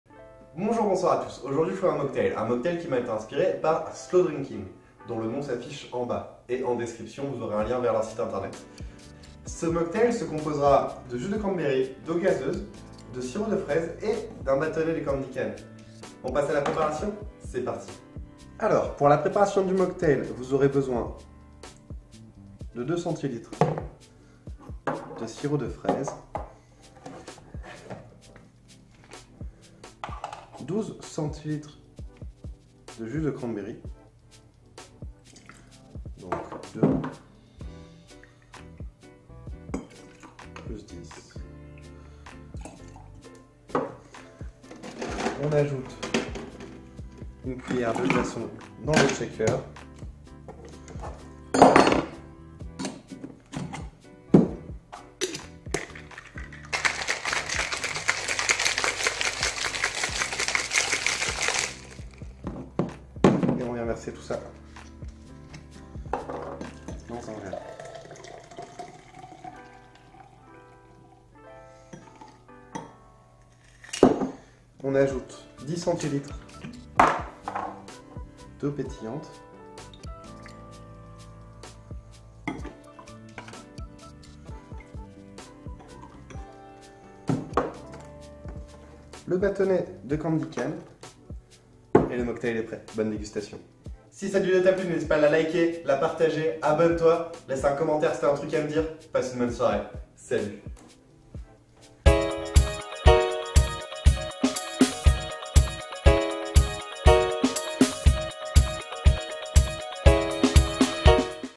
CANDY CANE SPARKLER - Mocktail
Voici un mocktail qui s'inspire de noël et que j'ai trouvé grâce à slow-drinking Je vous souhaite une bone dégustation :D